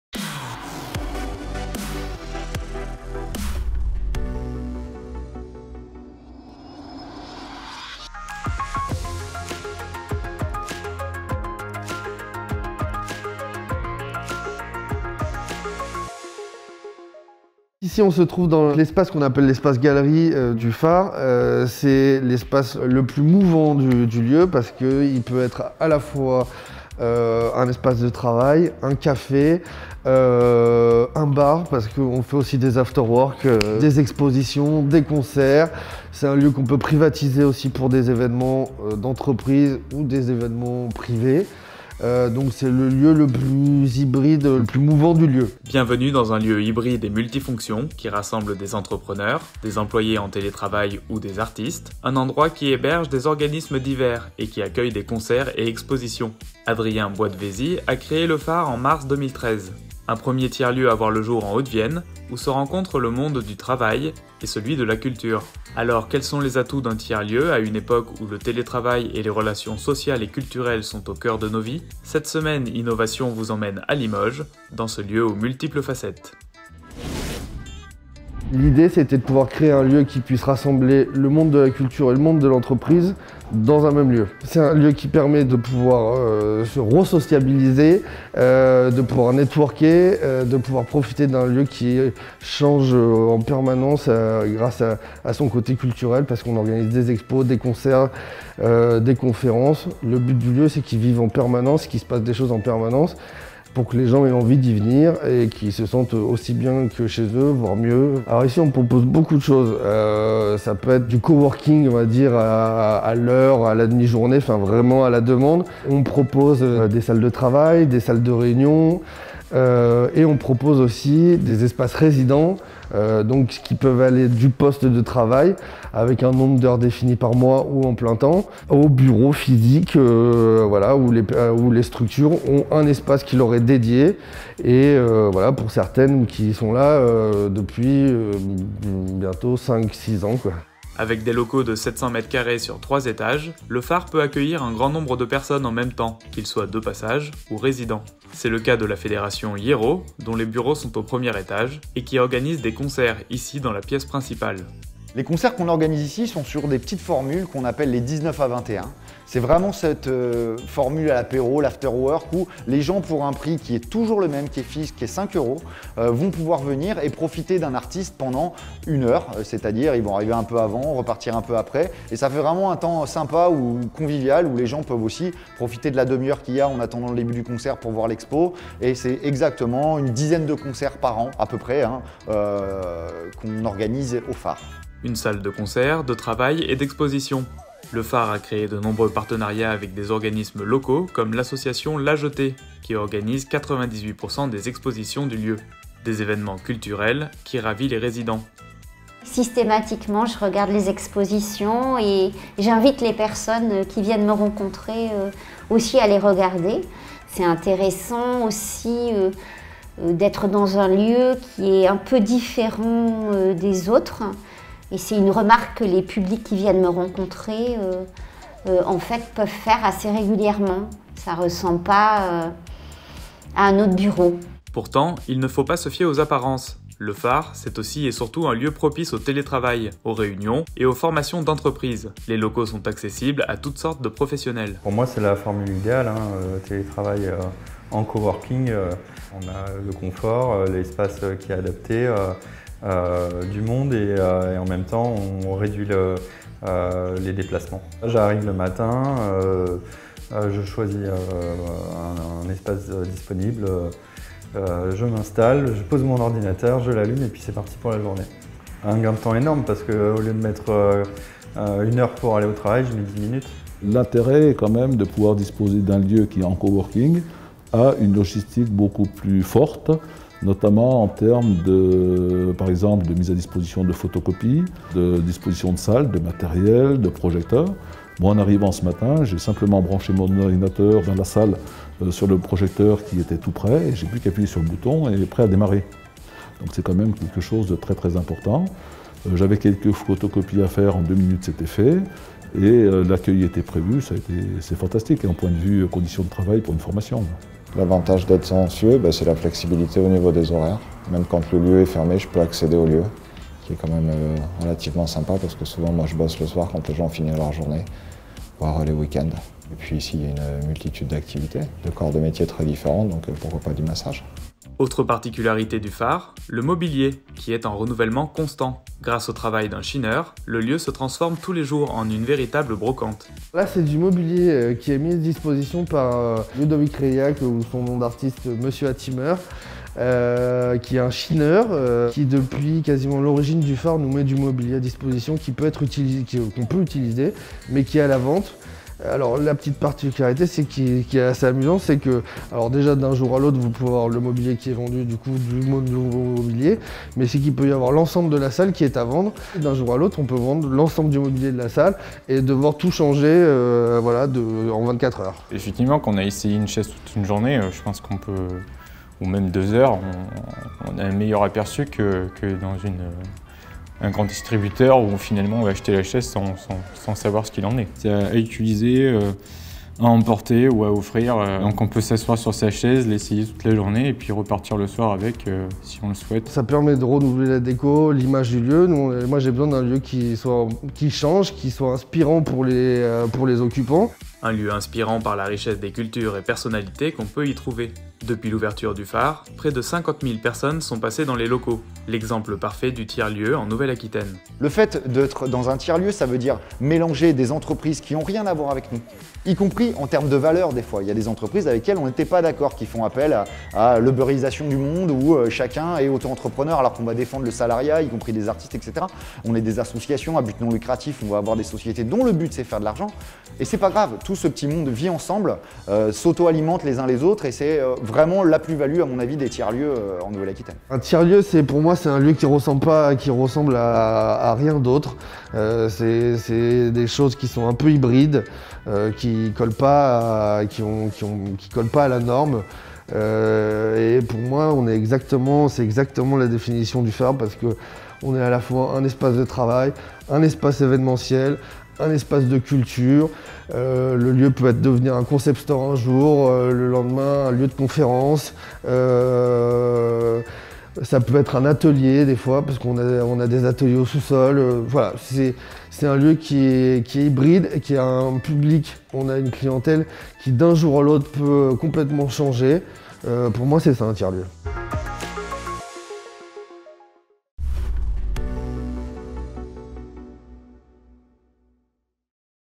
Innovations - Le Phare : rencontre entre le monde de la culture et de l'entreprise
Adrien Boisdevesy a créé le Phare en mars 2013. Un premier tiers lieu à voir le jour en Haute Vienne où se rencontre le monde de l'entreprise et celui de la culture.

Adrien Boisdevesy , fondateur du Phare
Nathalie Gorce, Nicolas Mathout, Jean-Paul Delherme, Jérémy Galliot, Ludovic Reilhac